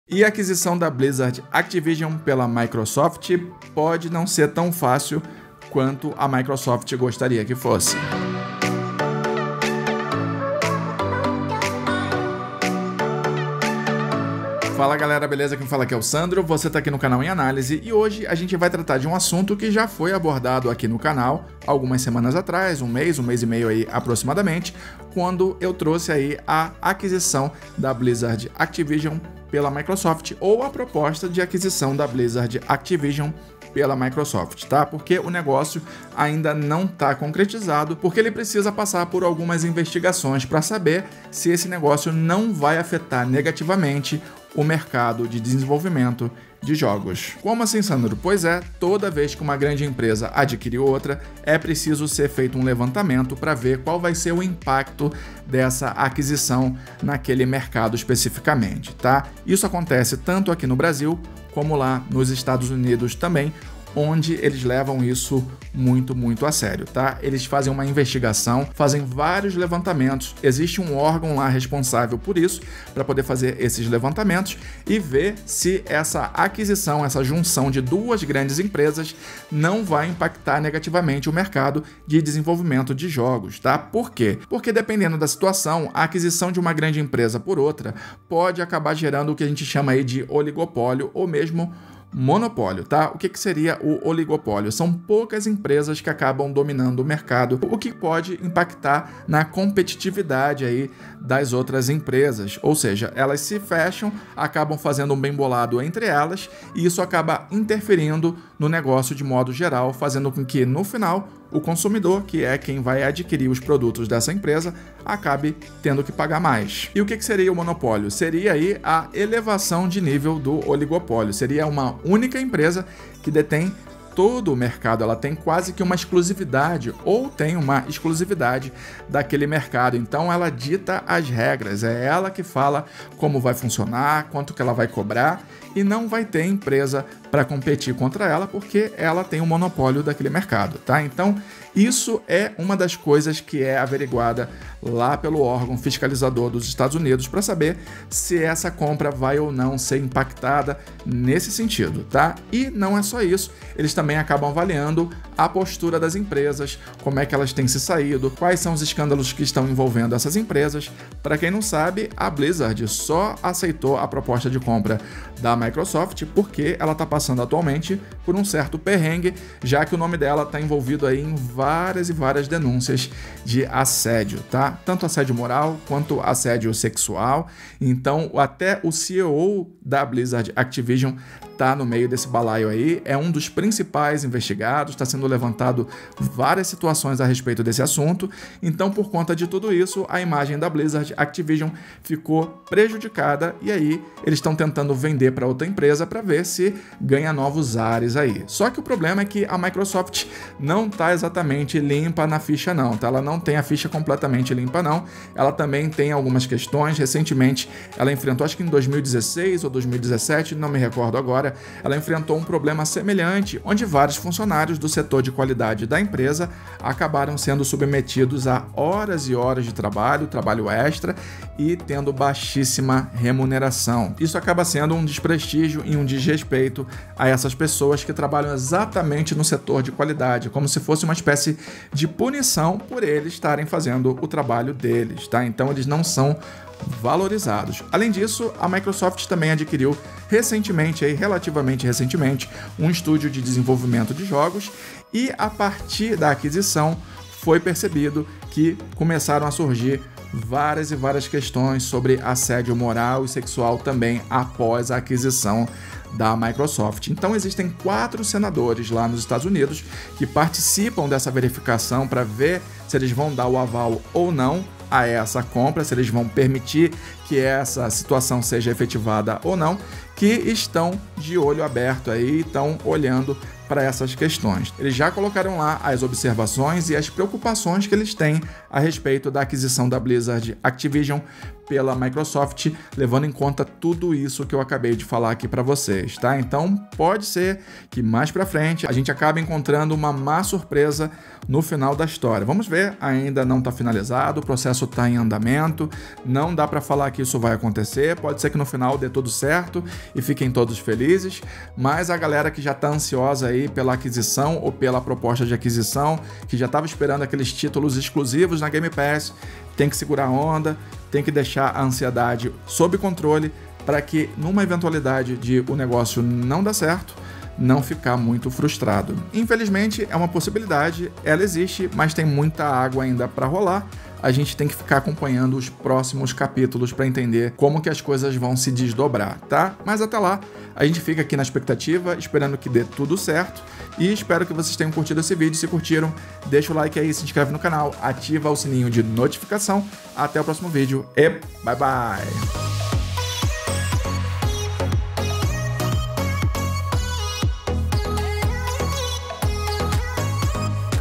A Microsoft Pode não Conseguir Concluir a Compra da Blizzard!
Recentemente foi noticiado a aquisição da Blizzard/Activision pela Microsoft, ou ao menos a proposta de aquisição, pois o negócio só pode ser fechado após os levantamentos do FTC (Federal Trade Commission, ou Comissão Federal de Comércio) serem concluídos.
E parece que já apareceram os primeiros possíveis problemas para a efetivação da compra da Blizzard pela Microssfot!
Aparentemente 4 senadores americanos já se mostram contrários a essa aquisição, o que pode impactar, e até mesmo impedir, que a compra da Blizzard/Actvision pela Microsoft seja concluída!

Equipamentos Utilizados na Gravação:

Câmera Canon SL2
Lente Canon 24mm F2.8
Microfone HyperX Quadcast S